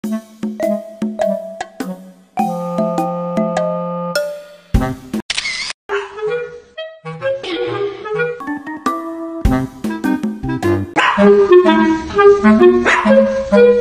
Adorable bichon is angry for its owner's sneaky shooting behavior
The clever bichon knows its owner is photo shooting to it sneaky. It is so angry about this behavior.